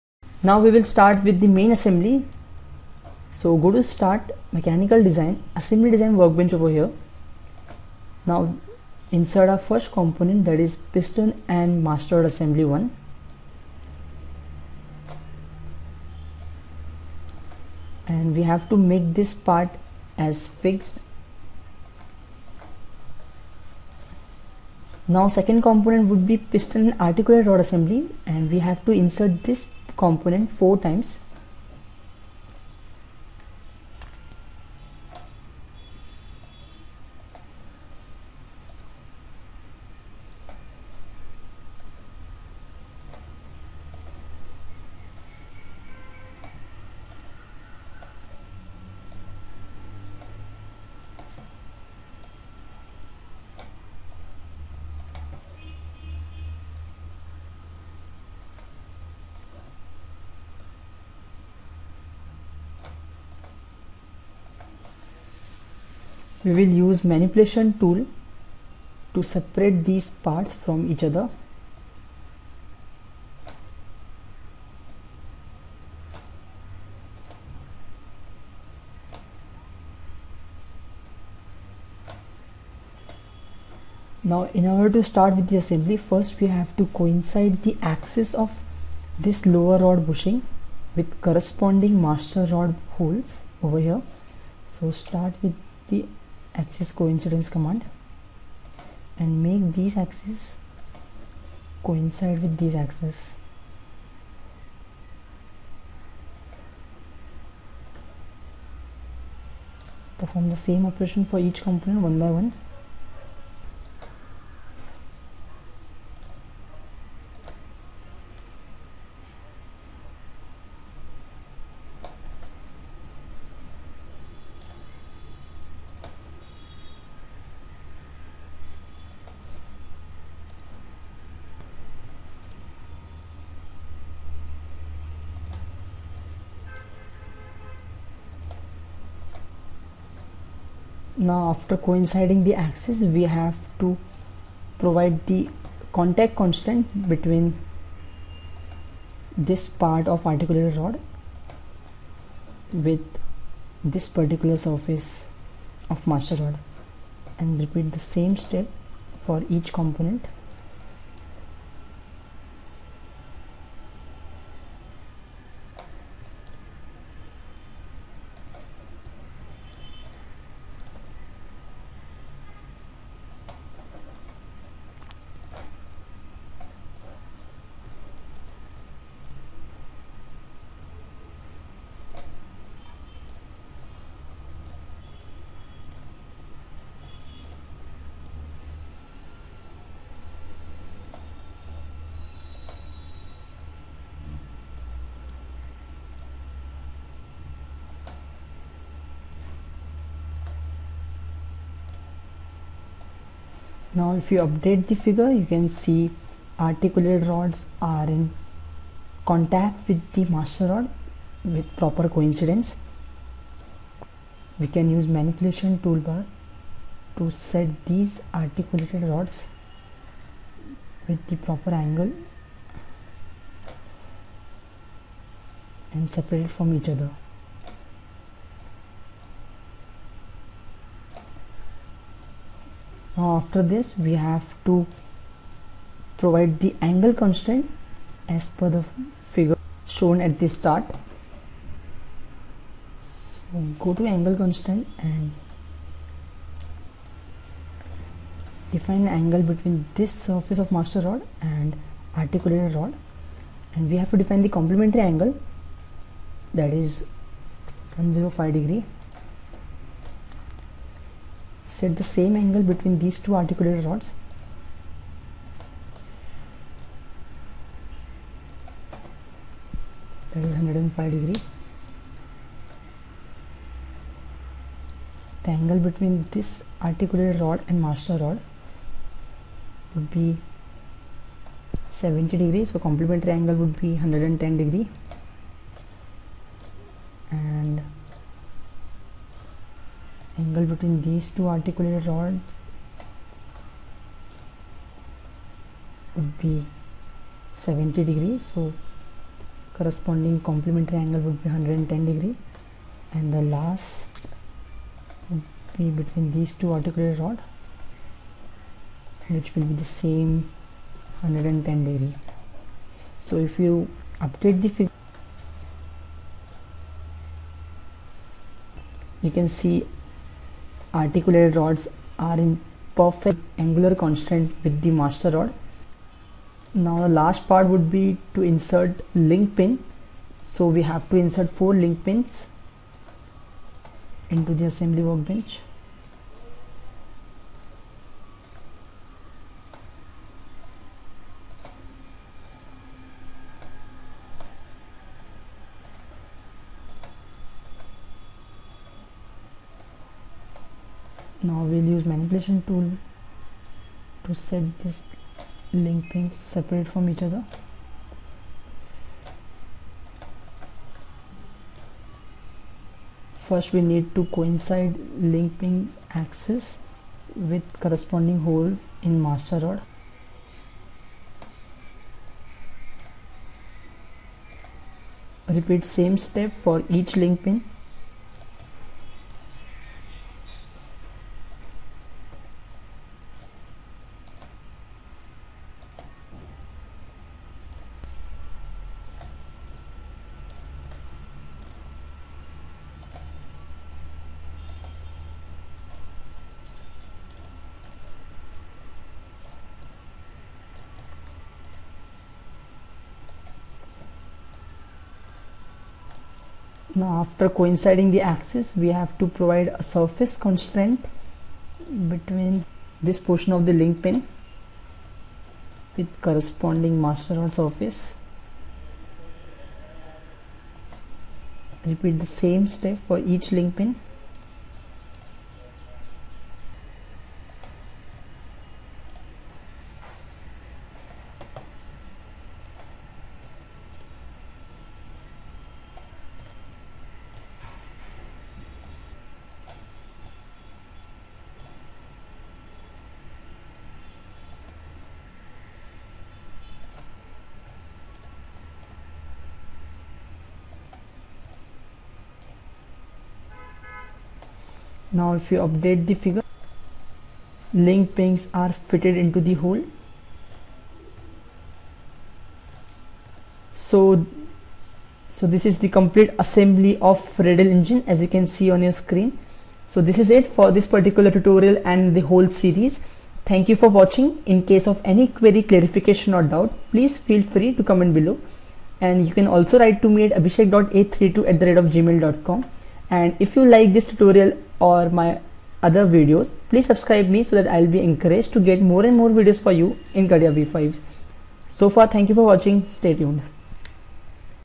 Catia Tutorials\How to create Radial Engine Assembly\Final Main Assembly Part_4.5
I have shown final main assembly of all sub assemblies to make completely assembled Radial Engine in Catia V5.

Mode:Beginner and Intermediate

Worksbench:
Assembly Workbench

Commands used:
1)Coincidence
2)Contact Constraint
3) Offset Constraint
4)Angle Constraint